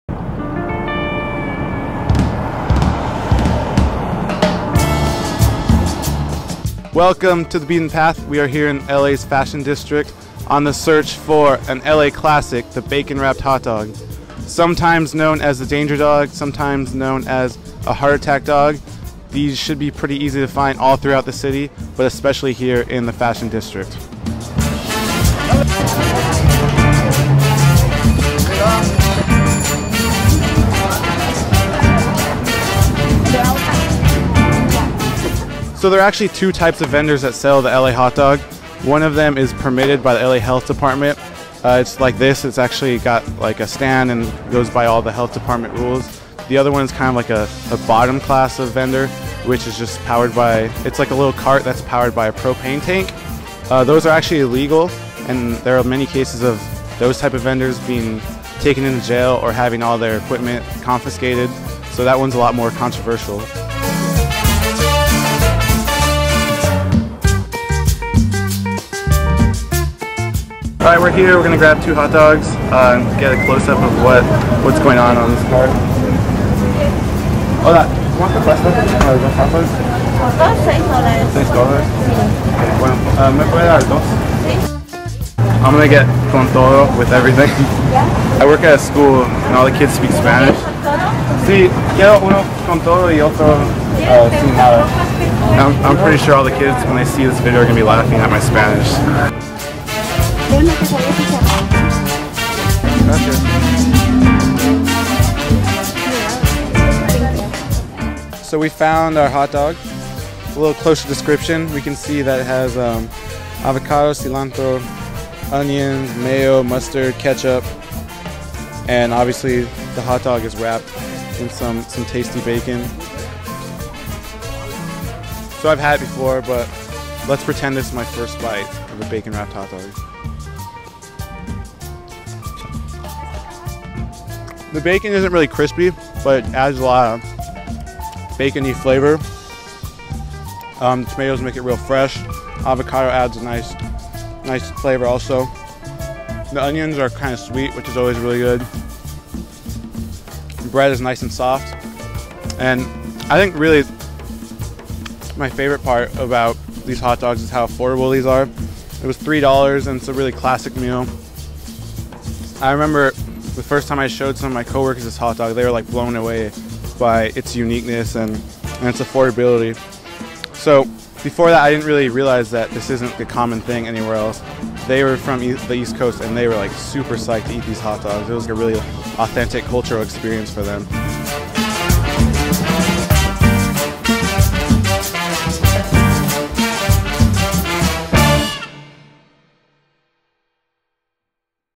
Los Angeles' Bacon Wrapped Hot Dog
In this episode we head to LA's fashion district in search of a local favorite, the bacon wrapped hot dog.